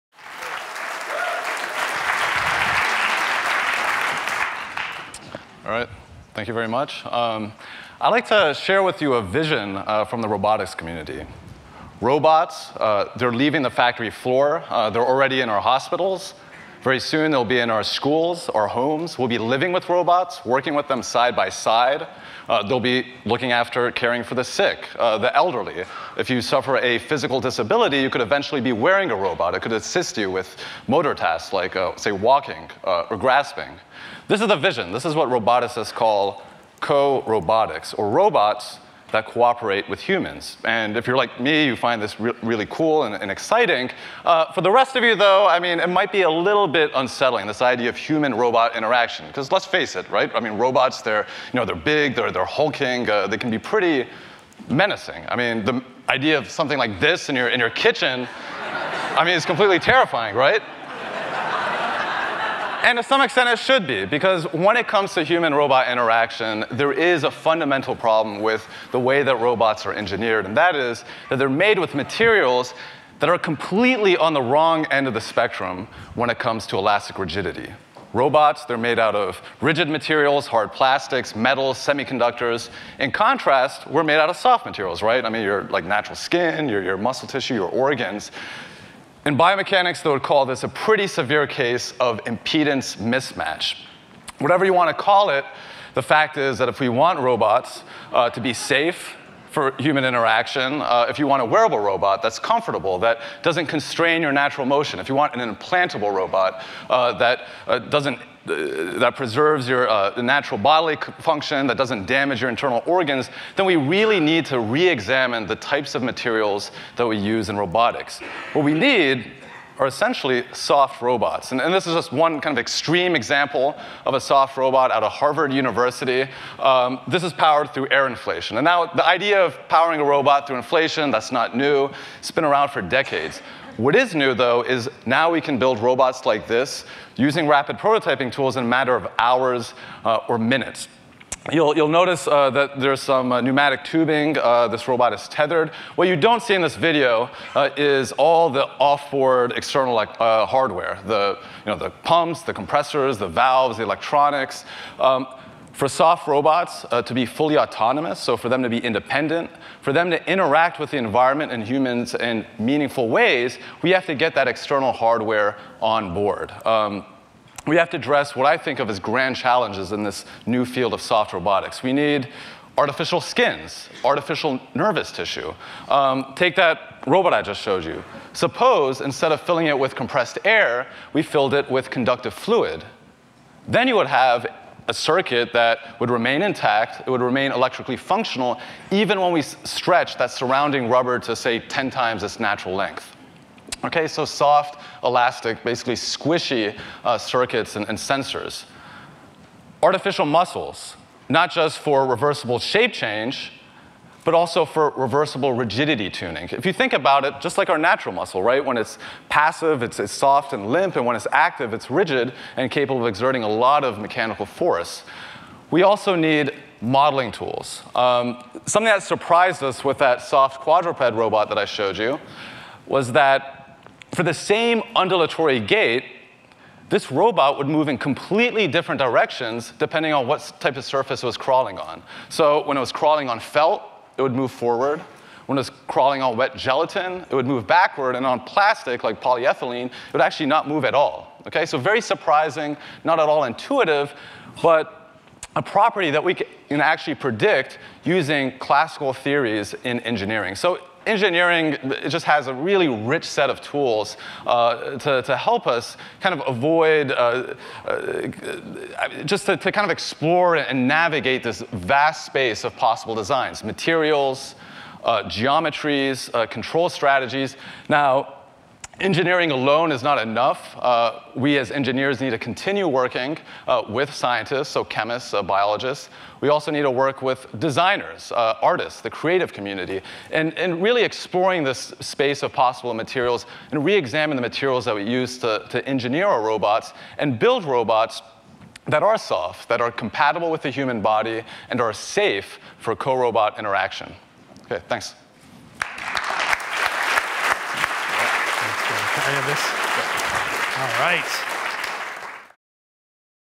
Carmel Majidi: Soft robots
Carmel Majidi discusses how the robots of the future will be pliable and soft to accommodate the characteristics of the human body. "We really need to reexamine the types of materials that we use in robotics."